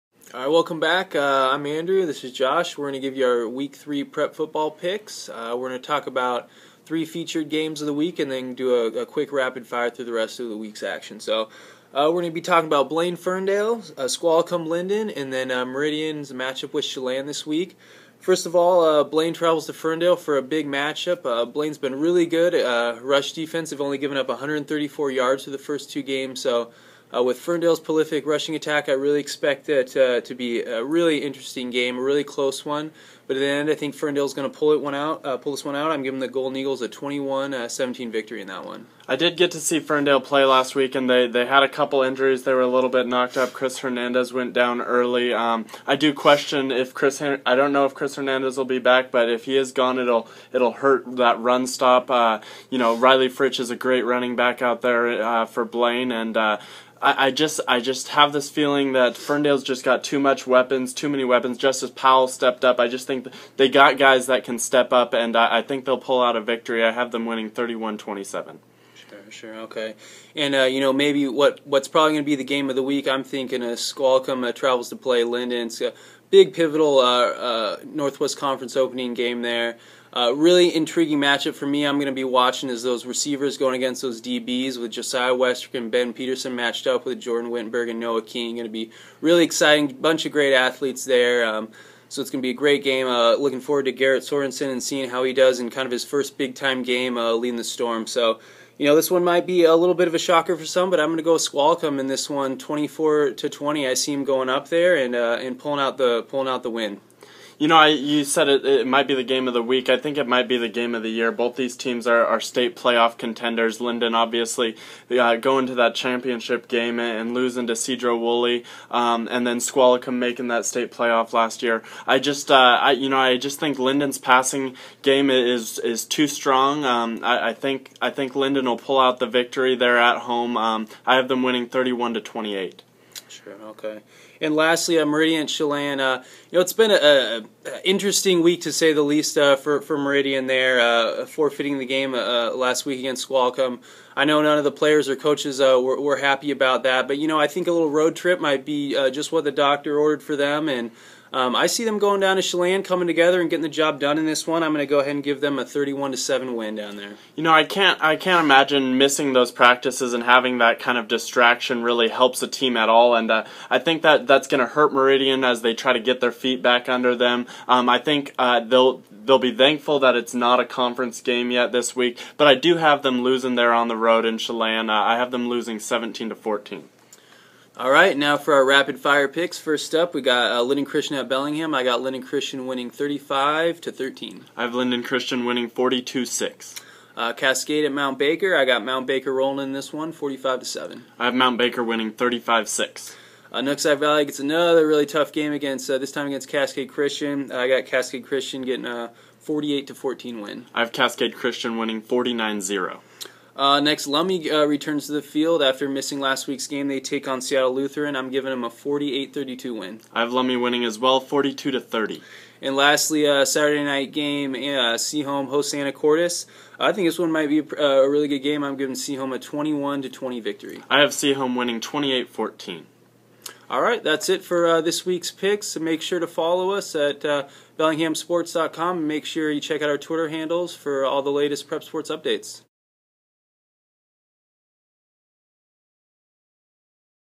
Week 3 Prep Football Picks
Sports writers Andrew Lang (@andyde44) and Joshua Hart (@JTHart23) discuss this week's highlighted games including Blaine, Ferndale, Squalicum, Lynden and Meridian and give their picks for all of this week's contests involving Whatcom County teams.